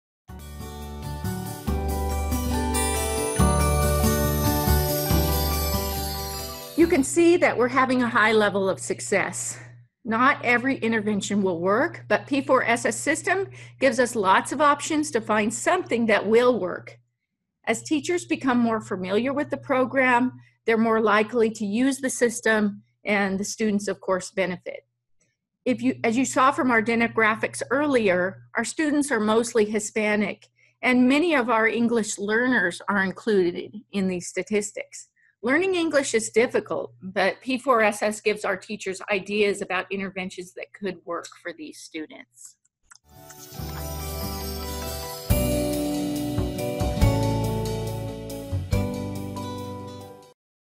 Partnering4StudentSuccess: Proven Success
Partnering4StudentSuccess (P4SS): Proven Success

Rhonda Cleeland and Cara Molina share Ontario-Montclair School District’s groundbreaking solution that has 86.5% (avg.) of their struggling students succeeding in their General Ed classrooms.